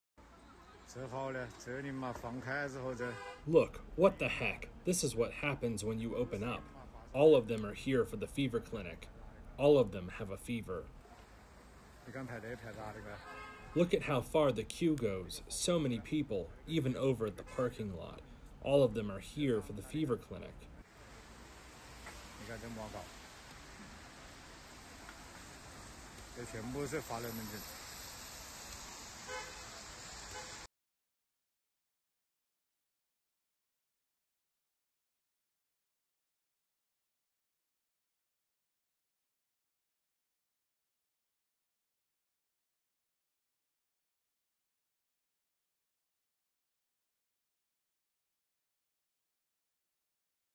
Mai multe persoane din Wuhan au făcut febră după ce regulile anti-COVID au fost relaxate
O coadă lungă s-a format în fața unei clinici de la un spital local din orașul Wuhan din centrul Chinei, la câteva zile după ce țara a relaxat unele dintre regulile stricte ale COVID-19, a arătat o înregistrare video difuzată vineri (9 decembrie) de martori oculari.

China a anunțat miercuri (7 decembrie) cele mai radicale modificări ale regimului său hotărât anti-COVID de la începutul pandemiei, în urmă cu trei ani, relaxând regulile care au limitat răspândirea virusului, dar care au stârnit proteste și au împiedicat a doua cea mai mare economie a lumii.

În înregistrarea video, realizată în orașul Wuhan, unde a apărut pentru prima dată virusul, se pot vedea cel puțin zeci de persoane care stau la coadă în fața unei clinici pentru febră, în timp ce un martor este auzit exclamând: "Asta se întâmplă când renunți la restricii! Toți sunt aici pentru că au febră!".

Potrivit presei de stat chineze, clinicile de febră au fost înființate pentru prima dată la nivel național în timpul epidemiei sars-COVID și servesc la separarea pacienților potențial infecțați de cei neinfectați.

Relaxarea regulilor, care permite persoanelor infectate cu simptome ușoare să stea în carantină la domiciliu și renunțarea la testele pentru persoanele care călătoresc în țară, este cel mai clar semn până acum că Beijingul se îndepărtează de politica sa de "zero-COVID" pentru a lăsa oamenii să trăiască cu boala.

Dar oficialii din domeniul sănătății avertizează în continuare că vor urmări îndeaproape tendințele în ceea ce privește numărul de decese și adecvarea resurselor medicale, în cazul în care va fi nevoie de o revenire la măsuri mai dure.